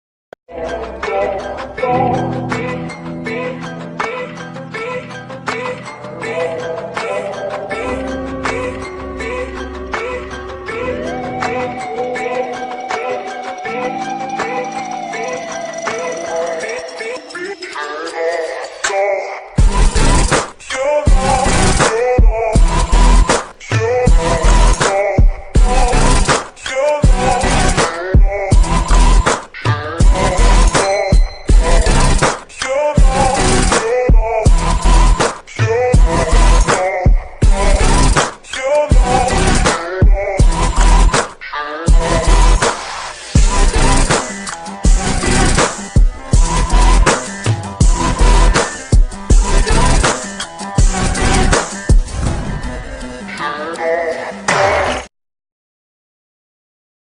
•you know• MEME - Stick Nodes
it's finally done, now to make heist meme .this is a sneak peek of holy and ye i am fighting raso too some parts might look off due to my rushness. i mean come on college is destroying my motives so here you go my meme animation, hope u will enjoy.

damn this boy is fast i was thinking of you know meme since 19 oct :thonk: